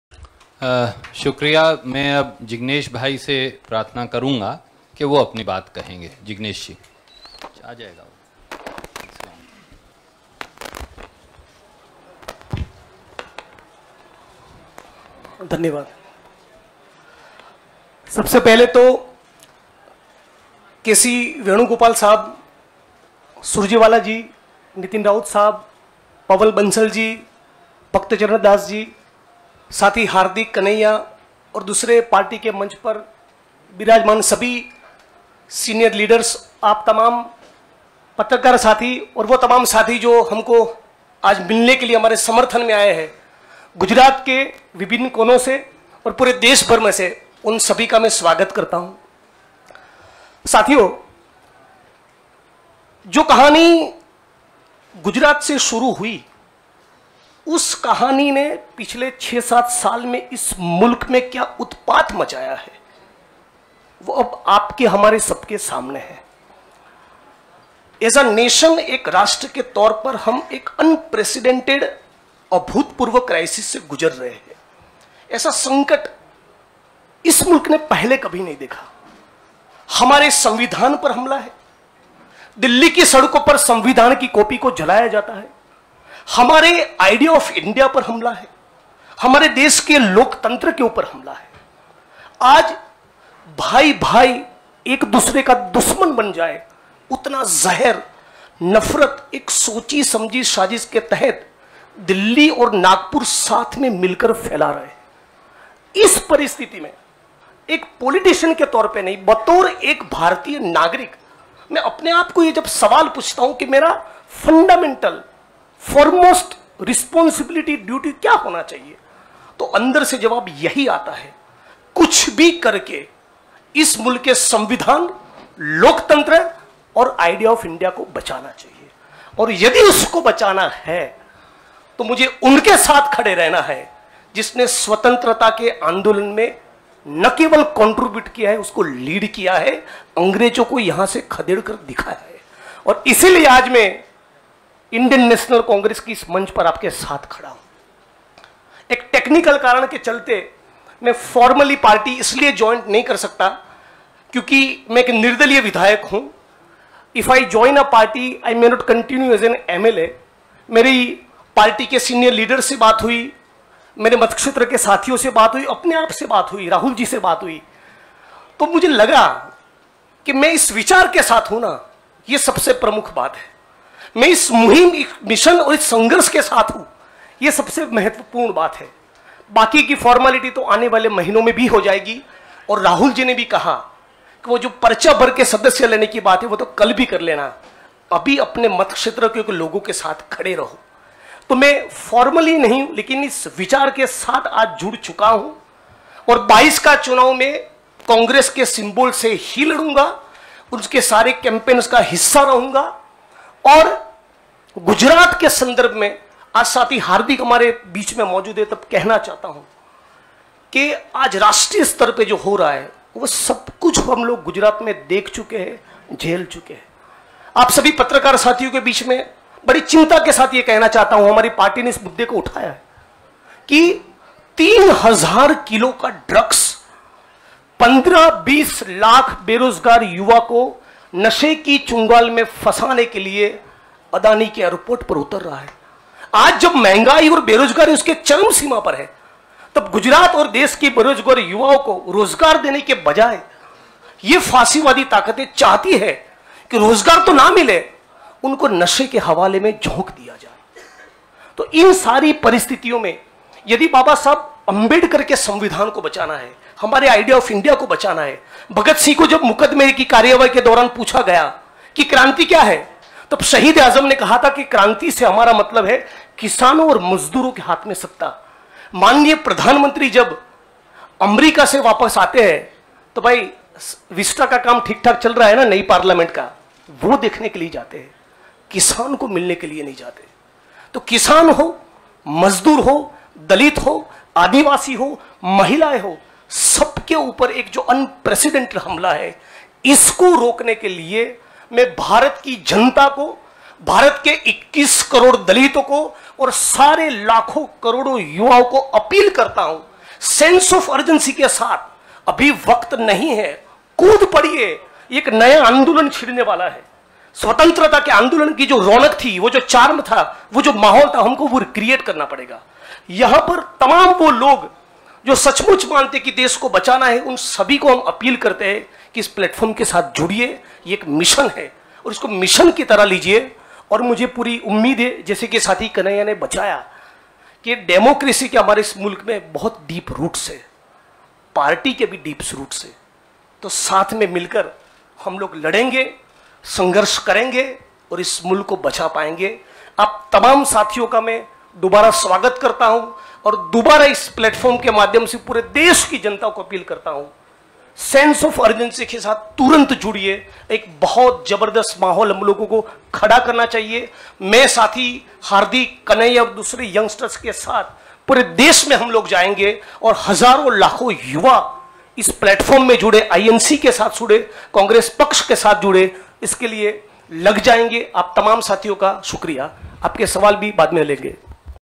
Jignesh Mevani Speech After Joining Congress Party in Delhi
LIVE: Special Press Conference by Kanhaiya Kumar, Jignesh Mevani, KC Venugopal, Randeep Surjewala and Bhakta Charan Das at AICC HQ. Earlier Comrade Kanhaiya Kumar and Gujarat MLA Jignesh Mewani meet Congress leader Rahul Gandhi at Shaheed-E-Azam Bhagat Singh Park, ITO, Delhi

🔴कांग्रेस में शामिल हुए कन्हैया कुमार और जिग्नेश मेवनी, दिल्ली से राहुल गाँधी की प्रेसवार्ता का सीधा प्रसारण LIVE देखिये !